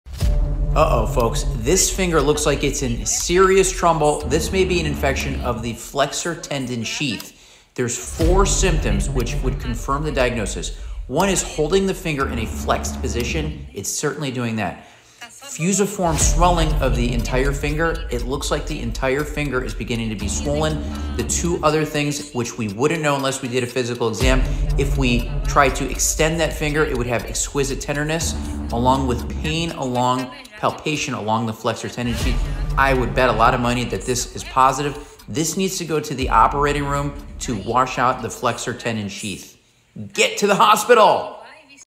Finger Infection!
If your finger has these types of symptoms, get to the emergency room!😷

😷✅Follow @DavidAbbasiMD for More Daily Medical Content & Health Tips!